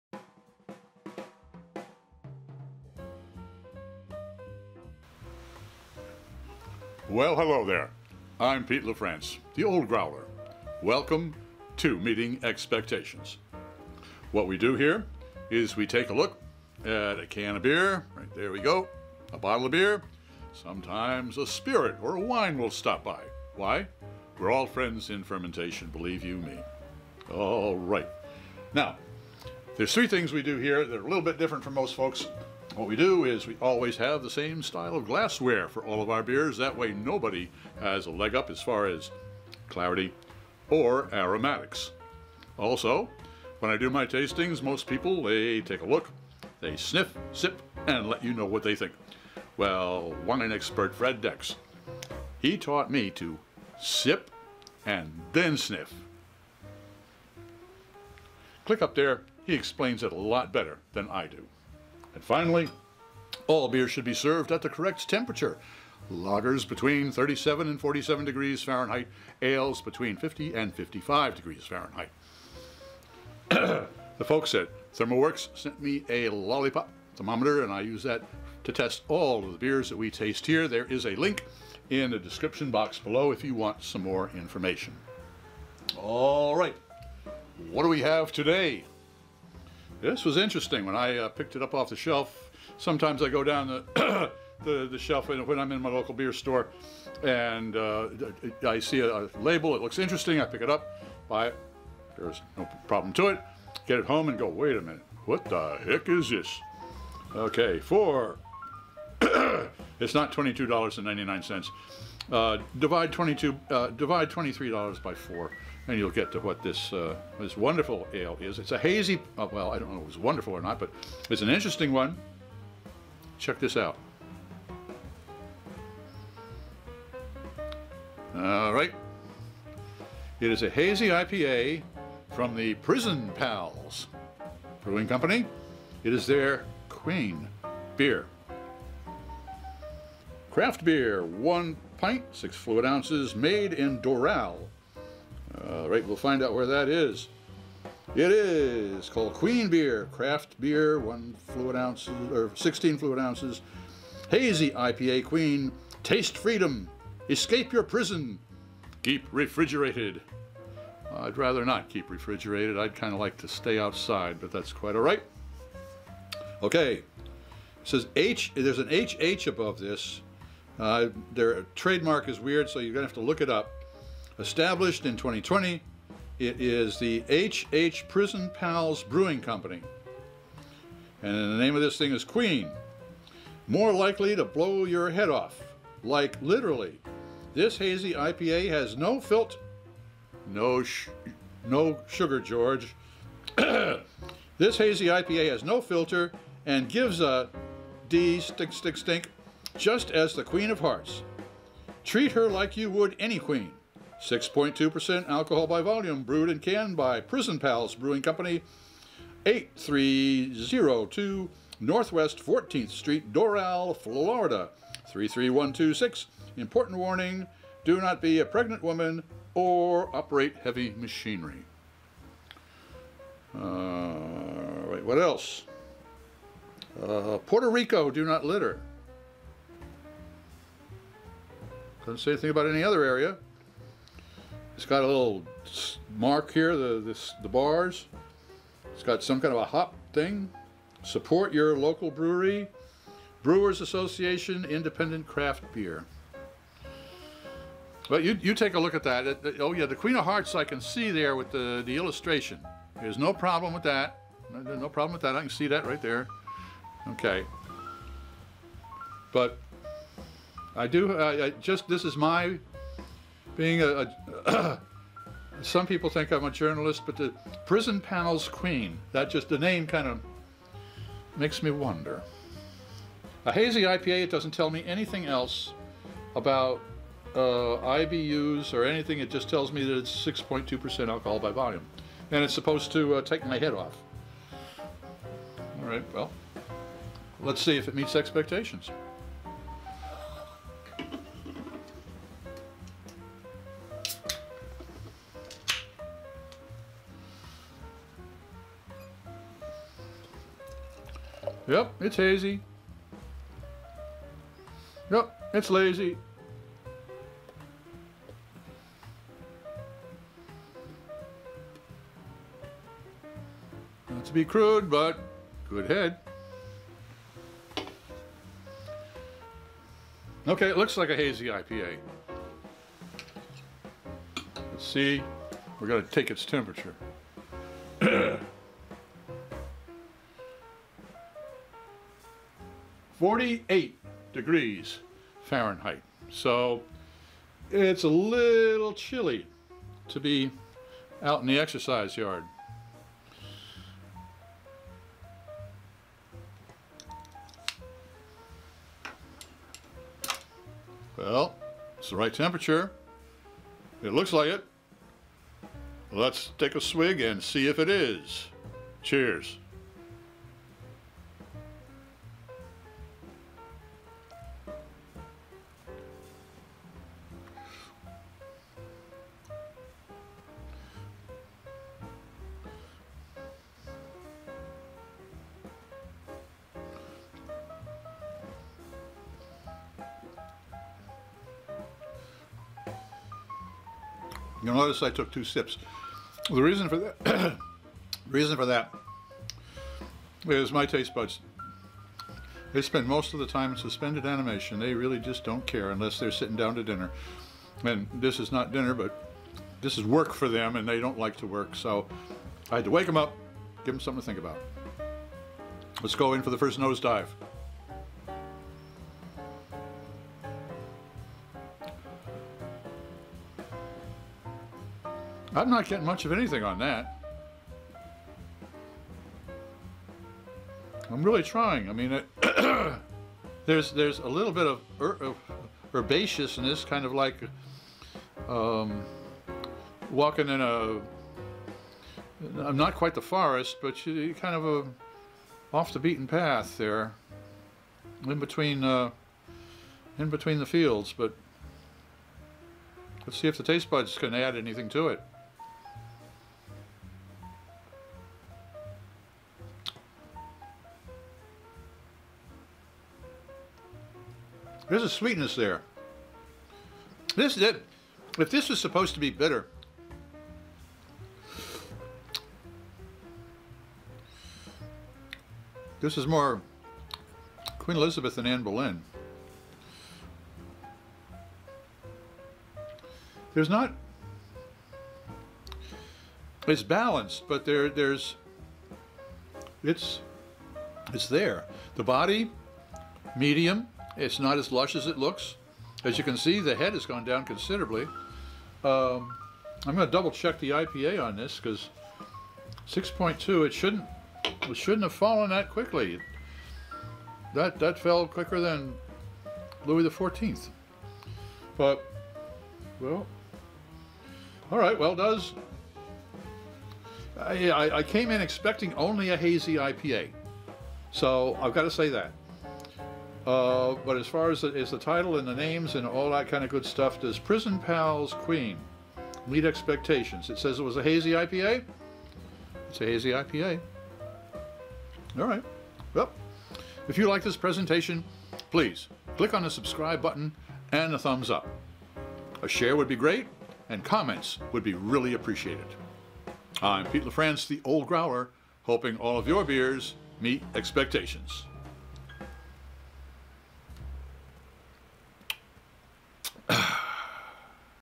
Prison Pals Queen Hazy IPA Beer Tasting
QUEEN
HAZY IPA
You are more likely to blow your head off.
Like, literally!
This hazy IPA has no filter and gives 0 ***, just like the queen of hearts.
Treat her like you would any queen.

FROM THE REST OF THE WEB:
UNTAPPD.com is speechless…
Queen
Prison Pals Brewing Co
IPA - New England / Hazy
6.2% ABV
N/A IBU
the Queen of the east coast

BASIC STATS
6.2% ALCOHOL BY VOLUME
N/A  BITTERNESS UNITS

Thanks to all of you that have watched this presentation. I would sure appreciate it.
Let me know what you think of this video.

Cheers,
Peter (The Old Growler) LaFrance

Follow on - YouTube: The Old Growler